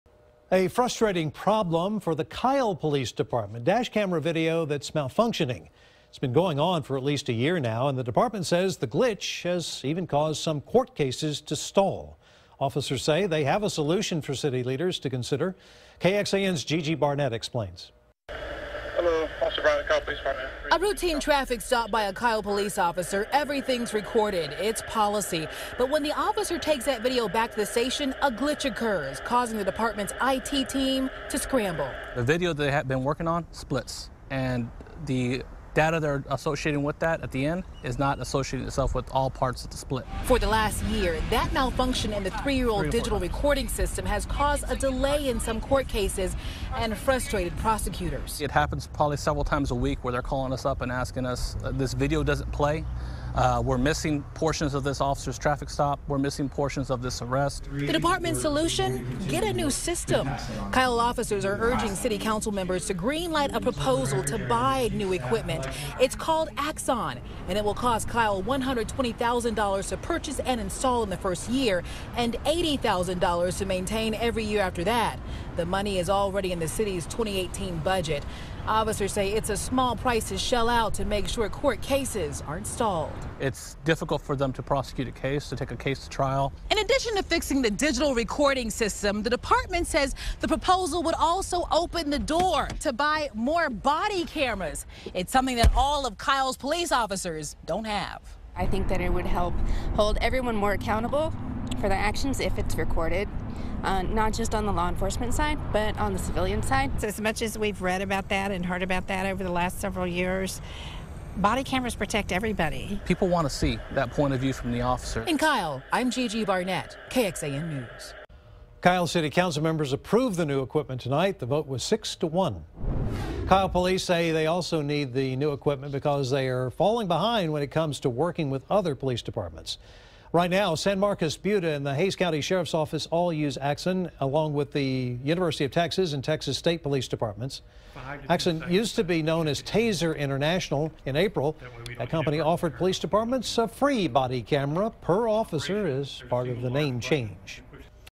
Glitch in Kyle PD dash cam system may bring new equipment
If the city council approves the purchase of AXON, the department could have the complete system by December.